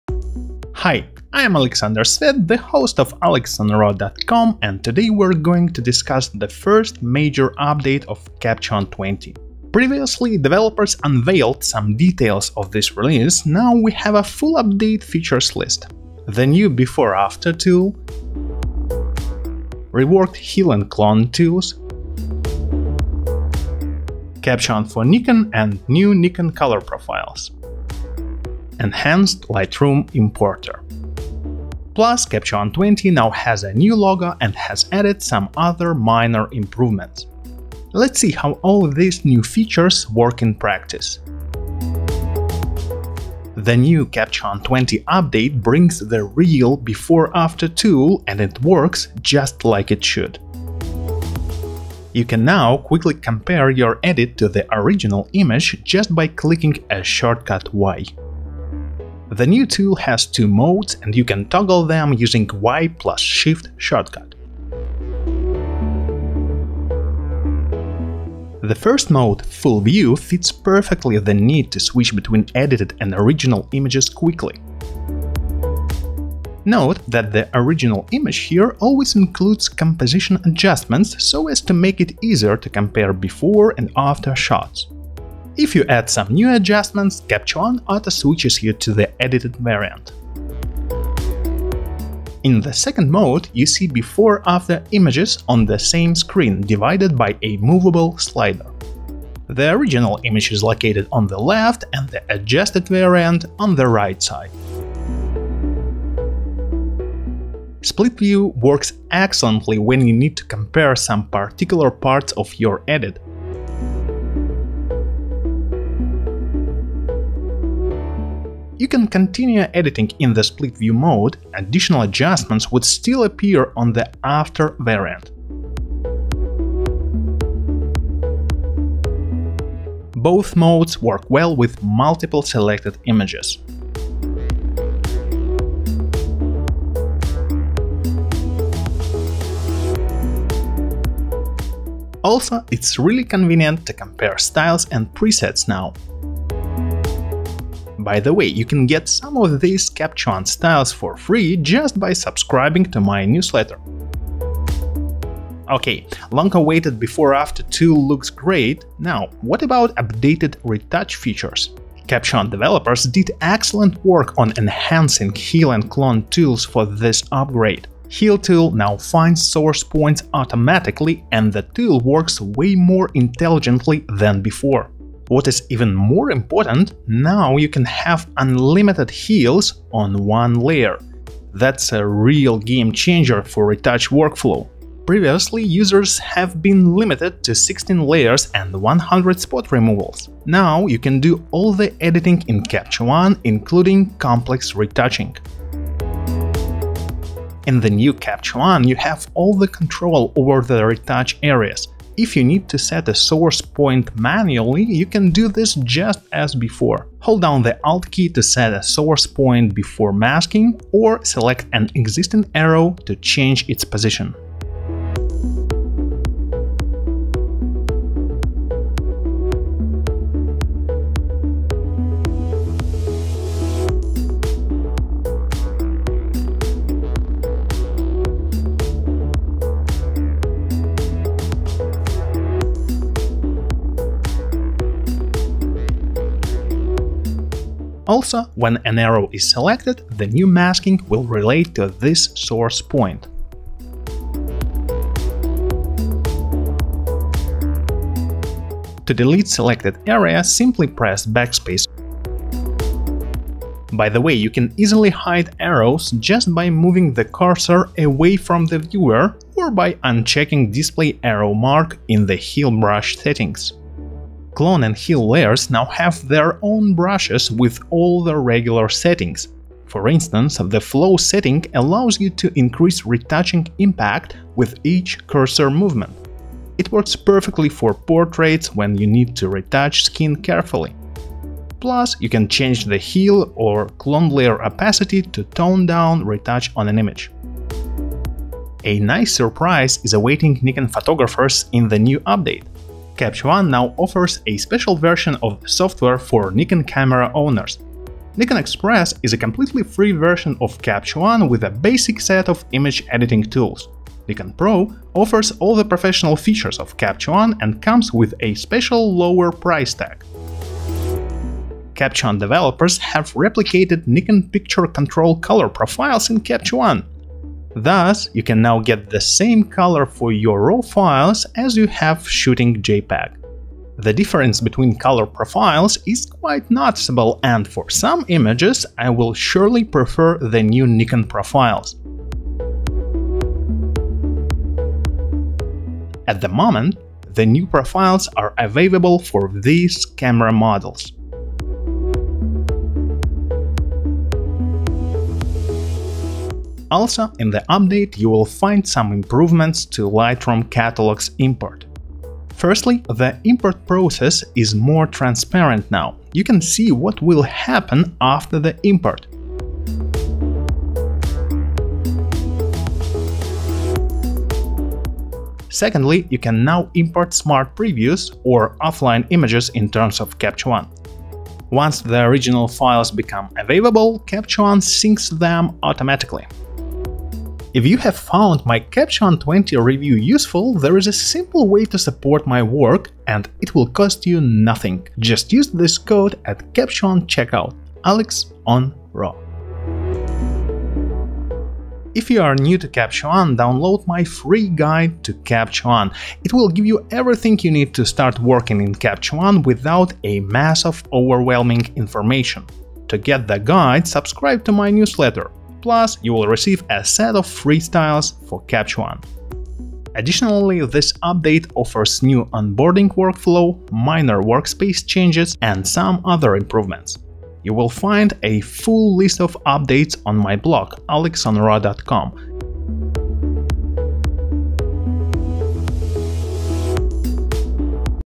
The New Capture One 20 Update: Before/After, Enhanced Retouching, and Capture One for Nikon
The first major update of Capture One 20 is finally here!
Previously, developers unveiled some details of this release; now we have a full update features list: New Before/After Tool, Reworked Heal and Clone tools, Capture One for Nikon and New Nikon Color Profiles, Enhanced Lightroom importer.

The New Before/After Tool
The new Capture One 20 update brings the real Before/After tool, and it works just like it should.
You can now quickly compare your edit to the original image just by clicking a shortcut Y.

Reworked Heal and Clone tools
Capture One developers did excellent work on enhancing Heal and Clone tools for this upgrade.
Heal tool now finds source points automatically, and the tool works way more intelligently than before.
What is even more important, now you can have unlimited heals on one layer. That's a real game-changer for retouch workflow.

Capture One for Nikon
Capture One now offers a special version of the software for Nikon camera owners.
Nikon Express is a completely free version of Capture One with a basic set of image editing tools.
Nikon Pro offers all the professional features of Capture One and comes with a special, lower price tag.

Enhanced Lightroom importer
Also, in the update, you'll find some improvements to Lightroom catalogs import.
Firstly, the import process is more transparent now; you can see what will happen after the import.
Secondly, you can now import Smart Previews (or Offline images in terms of Capture One). Once the original files become available, Capture One syncs them automatically.